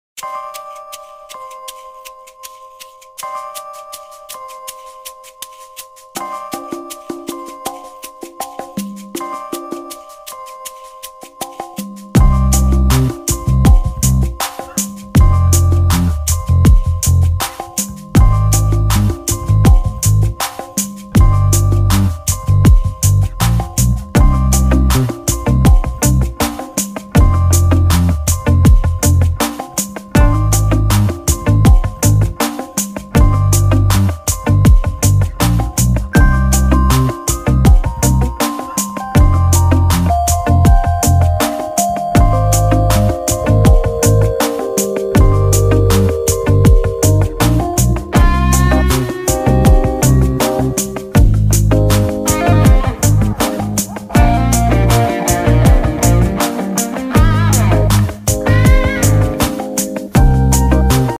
Queen cat breast feeding to her cute kittens
Either human or animals, a mother always cares for her baby, watch this beautiful cat breast feeding to her babies!